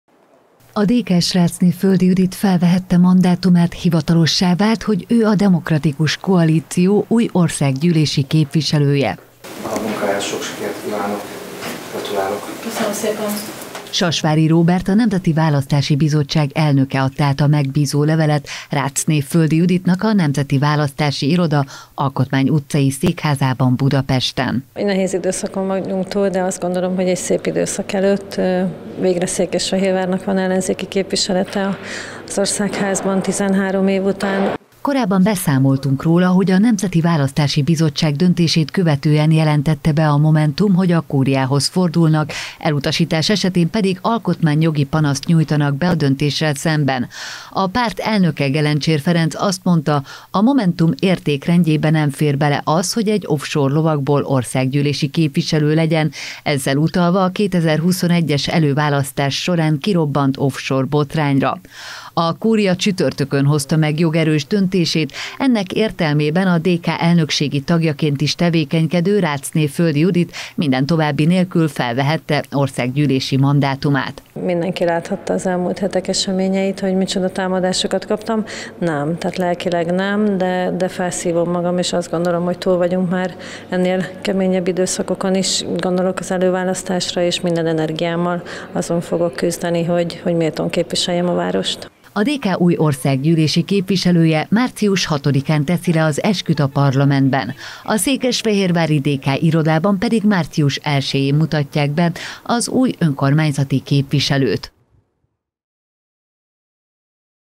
Átvette megbízólevelét
A Nemzeti Választási Bizottság döntése értelmében Ráczné Földi Judit lett a DK új országgyűlési képviselője. A politikus ma vette át megbízólevelét a Nemzeti Választási Bizottság elnökétől.